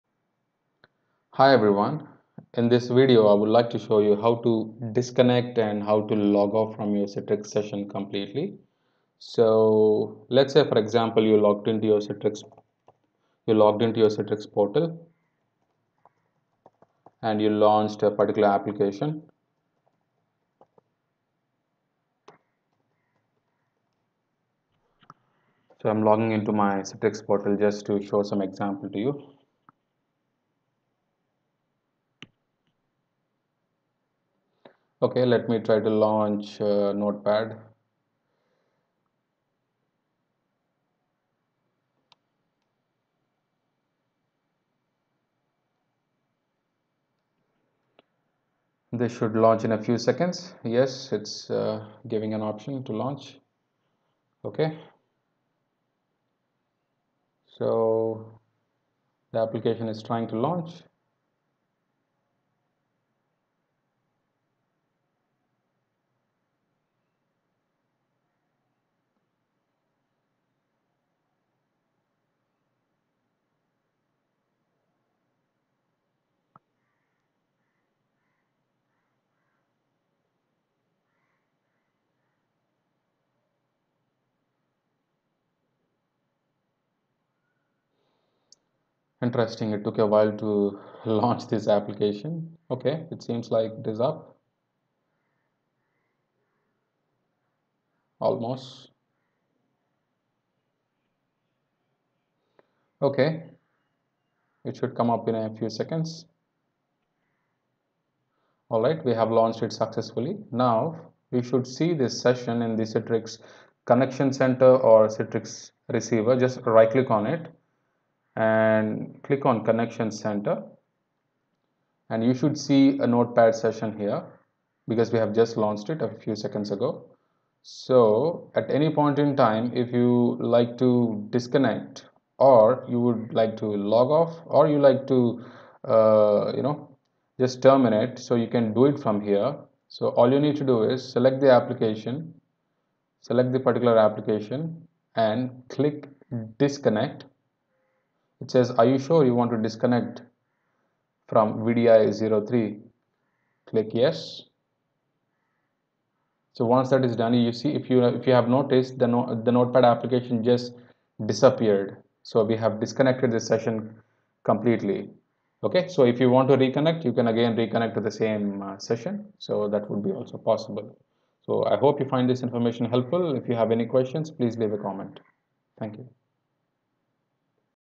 How to disconnect or logoff from citrix application
In this video, I showed you how we can logoff or disconnect from Citrix session. When you disconnect from your session, your session will be active on server, you may reconnect to the same session again later without any problems.

Thanks!